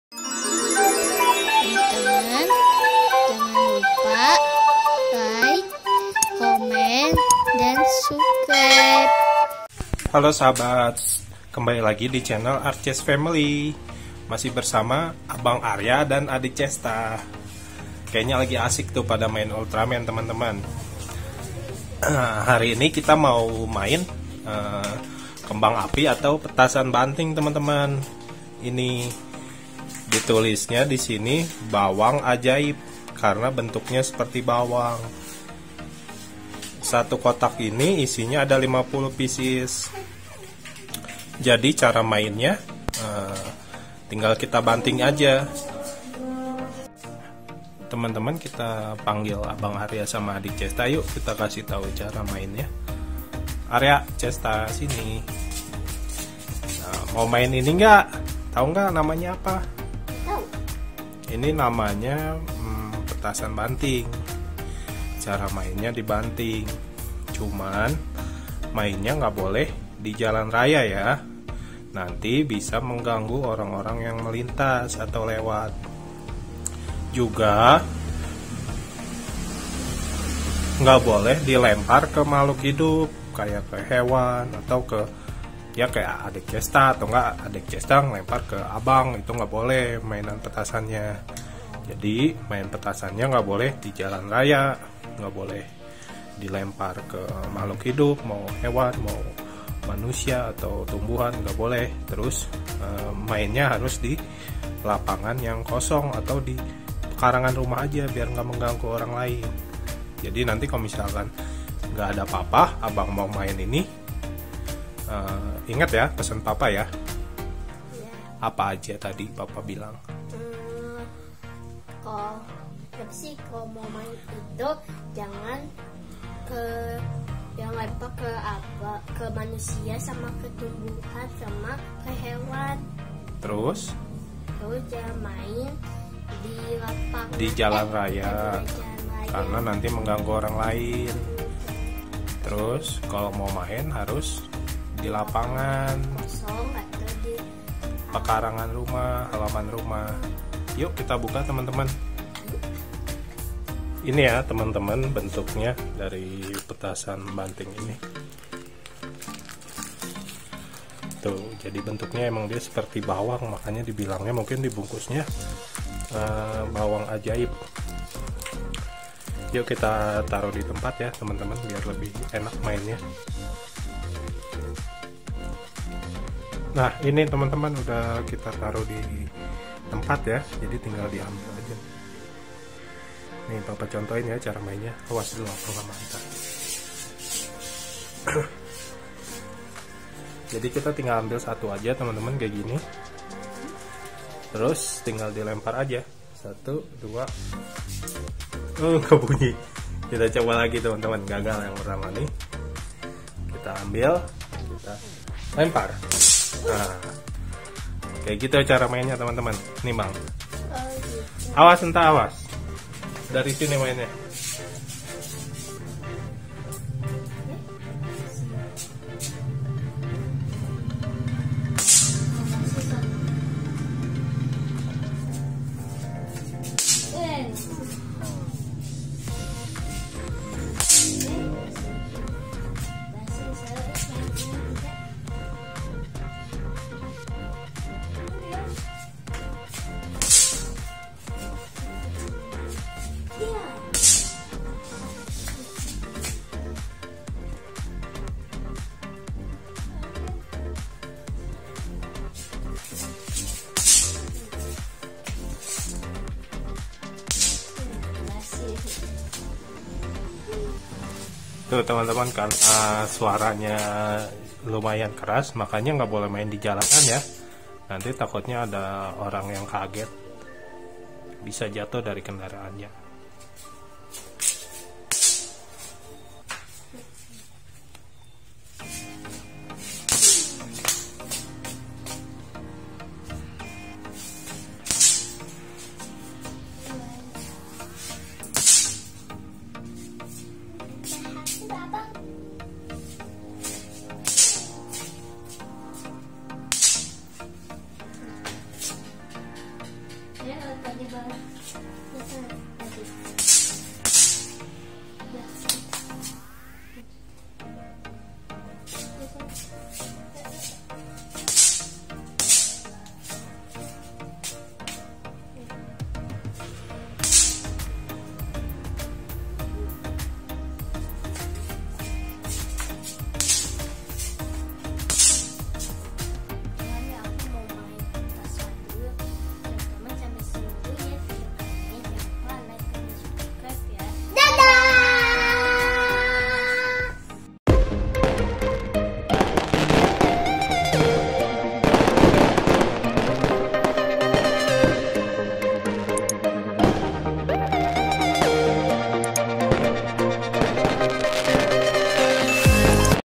funny kid's reaction the first time playing Petasan Banting | Pop Bawang | Arches Family
Teman - teman kali ini aku mau eksperiman atau mencoba pertama kali main petasan banting atau petasan bawang ada juga yang menyebut pop bawang, kalo di tempat kalian apa namanya ?

Selamat menonton.
Semoga terhibur
❤️ Terima kasih ❤️